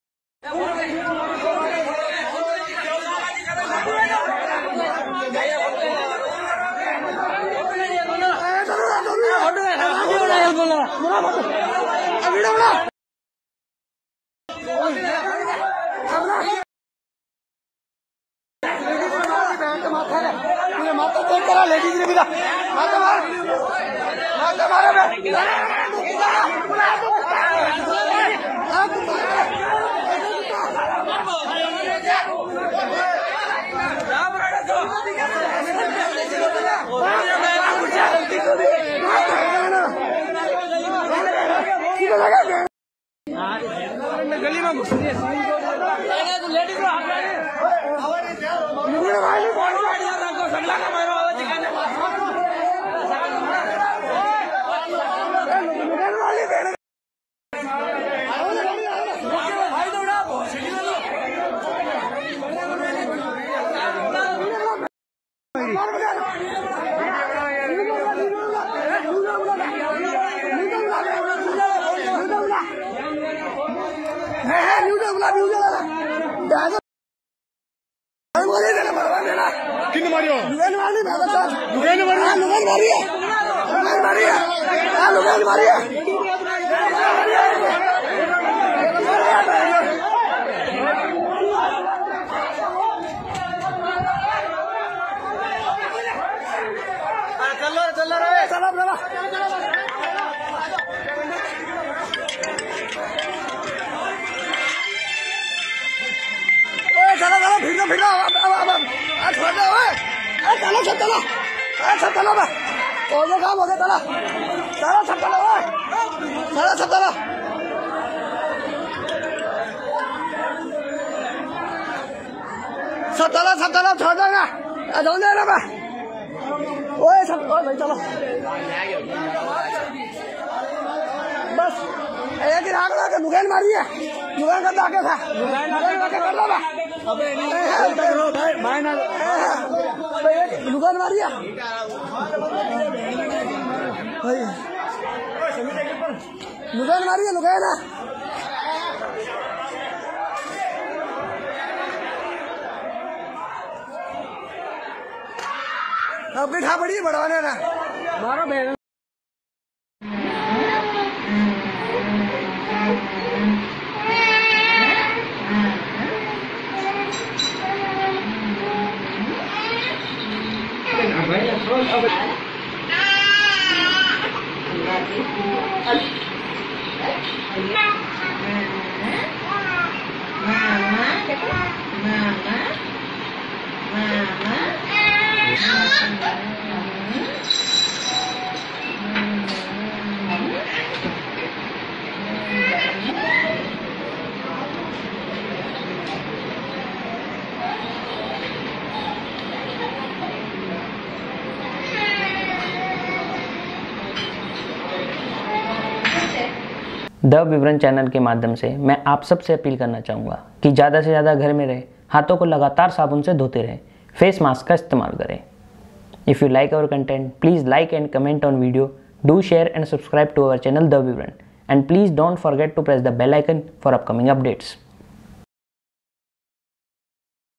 People attacking Police | पुलिस वालो के खिलाफ प्रदर्शन करते लोग | Coronavirus | The Vivran
Watch the video of people attacking and protesting against police.

Video Courtesy: Online Platform (Facebook/Instagram)
Editing Courtesy : Rajnish Bhatt
Produced by: The Vivran